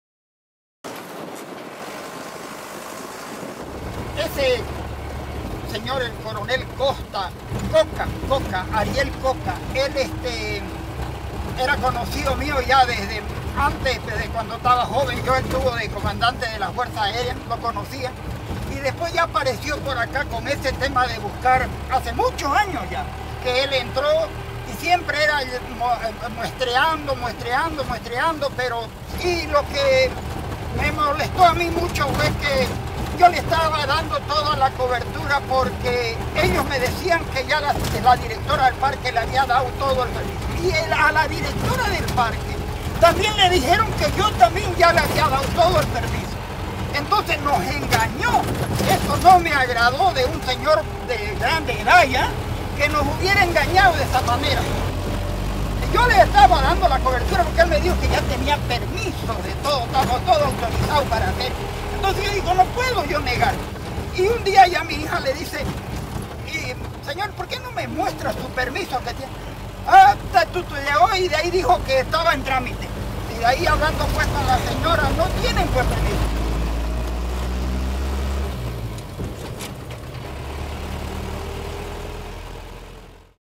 BOL377- Don Picky talks about mining at Kaa Iya 1
Don Luis Tipze Futner, neighbor of the Kaa Iya National Park, speaks about mining at the protected area.